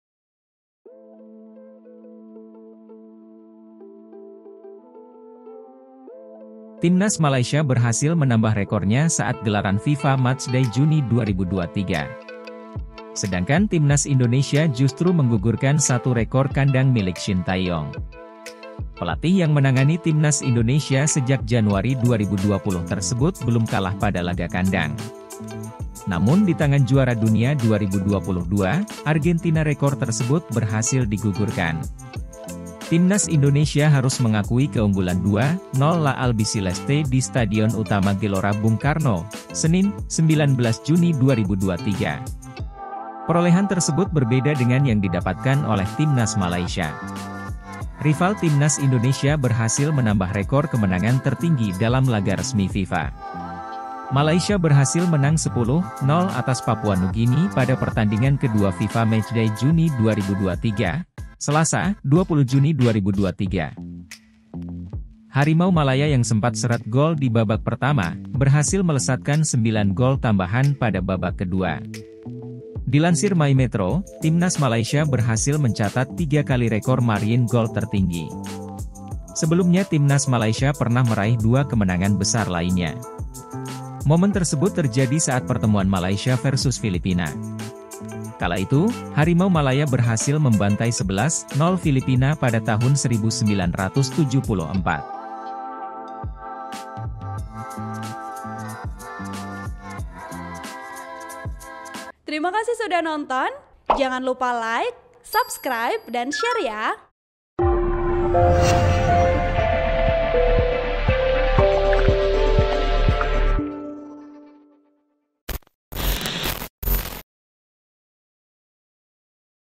Beda Nasib dengan Timnas Indonesia, Harimau Malaya Justru Sukses Tambah Rekor di FIFA Matchday
TRIBUN-VIDEO.COM - Timnas Malaysia berhasil menambah rekornya saat gelaran FIFA Matchday Juni 2023.

Sedangkan Timnas Indonesia justru menggugurkan satu rekor kandang milik Shin Tae-yong.

Pelatih yang menangani Timnas Indonesia sejak Januari 2020 tersebut belum kalah pada laga kandang.

Namun di tangan Juara Dunia 2022, Argentina rekor tersebut berhasil digugurkan.

Timnas Indonesia harus mengakui keunggulan 2-0 La Albiceleste di Stadion Utama Gelora Bung Karno, Senin (19/6/2023).

Perolehan tersebut berbeda dengan yang didapatkan oleh Timnas Malaysia.

Lantas, rekor apa yang mampu ditambah Harimau Malaya? simak ulasannya sebagai berikut.

Rekor Timnas Malaysia

Rival Timnas Indonesia berhasil menambah rekor kemenangan tertinggi dalam laga resmi FIFA.

Malaysia berhasil menang 10-0 atas Papua Nugini pada pertandingan kedua FIFA Matchday Juni 2023, Selasa (20/6/2023).

Harimau Malaya yang sempat seret gol di babak pertama, berhasil melesatkan sembilan gol tambahan pada babak kedua.

Pemain 21 tahun, Arif Aiman menjadi Man Of The Match dengan total empat gol dan dua assist.

Kemenangan di Stadion Sultan Mizan Zainal Abidin menambah rekor kemenangan Timnas Malaysia sepanjang masa.

Dilansir My Metro, Timnas Malaysia berhasil mencatat tiga kali rekor margin gol tertinggi.

Sebelumnya Timnas Malaysia pernah meraih dua kemenangan besar lainnya.

Momen tersebut terjadi saat pertemuan Malaysia vs Filipina.

Kala itu, Harimau Malaya berhasil membantai 11-0 Filipina pada tahun 1974.

Laga Malaysia vs Filipina terjadi saat Piala Asia yang digelar di Taheran, Irak.

Turnamen tersebut mengantarkan Malaysia bertengger di peringkat ketiga akhir turnamen.

Satu lagi kemenangan terbesar Malaysia ialah saat Olimpiade 1976 di Jakarta.

Kala itu, Malaysia bertemu juga dengan Papua Nugini dan berhasil menang dengan skor 10-1.

Adapun selain kemenangan tersebut, beberapa margin besar juga pernah diraih Malaysia.

Di antaranya saat menang 9-0 atas Kamboja di SEA Games 1995 Thailand.

Malaysia juga pernah mengalahkan Myanmar (dulu Burma) dengan skor 9-1 saat SEA Games 1977 Brunei Darrusalam.

Di balik rekor cemerlang, Malaysia pernah kandas 10-0 atas Uni Emirat Arab (UAE) saat Kualifikiasi Piala Dunia 2018.

Namun rekor buruk tersebut kini telah tergantikan oleh kemenangan besar pada pertandingan terakhir.

Selain menupas perlawan Papua Nugini, Timnas Malaysia juga berhasil menang pada laga pertama FIFA Matchday.

Malaysia menang 4-1 atas Kepulauan Solomon pada Rabu (14/6/2023).

Rekor Kandang Shin Tae-yong

Shin Tae-yong berhasil mengarungi 12 laga kandang tanpa kekalahan.

Namun, saat FIFA Matchday kali ini rekor tersebut berakhir.

Simak deretan rekor laga kandang Shin Tae-yong

27/1/2022 - Indonesia 4-1 Timor Leste (FIFA Matchday)

30/1/2022 - Indonesia 3-0 Timor Leste (FIFA Matchday)

1/6/2022 - Indonesia 0-0 Bangladesh (FIFA Matchday)

24/9/2022 - Indonesia 3-2 Curacao (FIFA Matchday)

27/9/2022 - Indonesia 2-1 Curacao (FIFA Matchday)

23/12/2022 - Indonesia 2-1 Kamboja (Piala AFF)

29/12/2022 - Indonesia 1-1 Thailand (Piala AFF)

6/1/2023 - Indonesia 0-0 Vietnam (Piala AFF)

25/3/2023 - Indonesia 3-1 Burundi (FIFA Matchday)

28/3/2023 - Indonesia 2-2 Burundi (FIFA Matchday)

14/6/2023 - Indonesia 0-0 Palestina (FIFA Matchday)

19/6/2023 - Indonesia 0-2 Argentina (FIFA Matchday)

(Tribunnews.com/Bayu Panegak/Ipunk)

Artikel ini telah tayang di Tribunnews.com dengan judul Berbeda dengan Timnas Indonesia, Harimau Malaya Sukses Tambah Rekor di FIFA Matchday,
Penulis: Bayu Satriyo Panegak